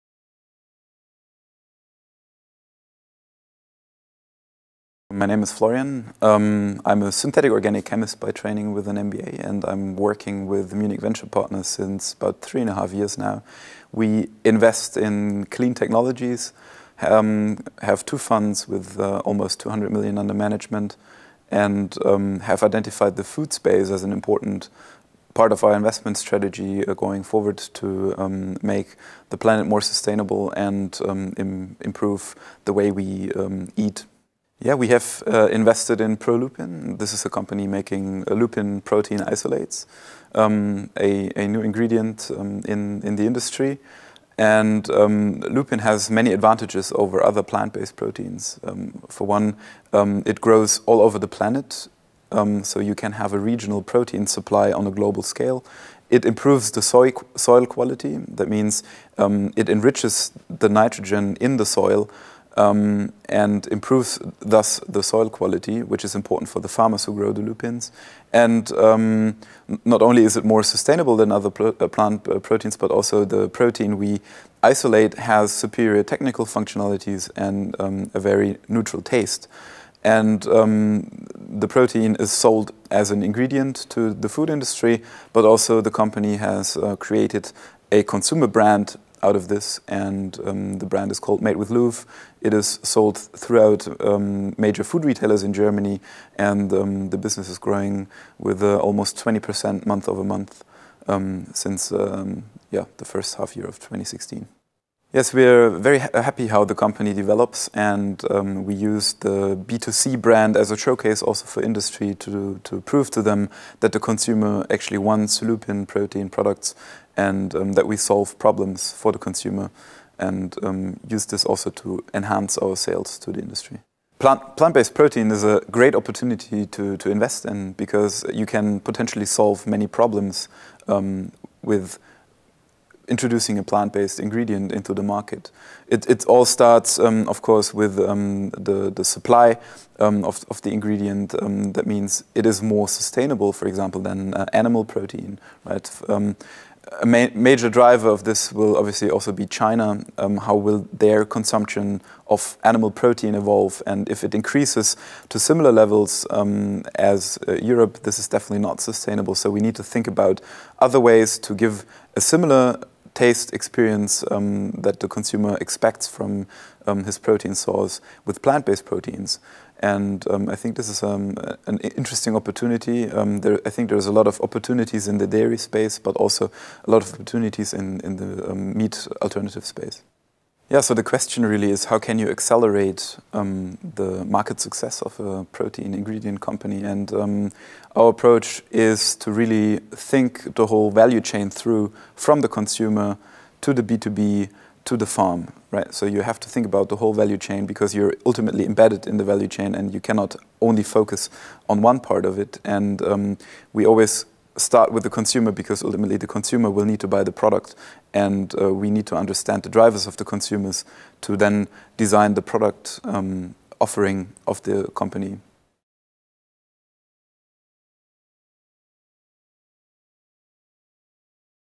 Florian Loebermann, Partner, Munich Venture Partners (Germany)
Munich Ventures invest in clean technology (£200million) identified food space as Investment strategy. Lupin is an excellent protein source - sustainable, enriches soil, and protein isolate of superior technical functionality.. Start with consumers and need to think whole value chain from consumer to business to business to farm.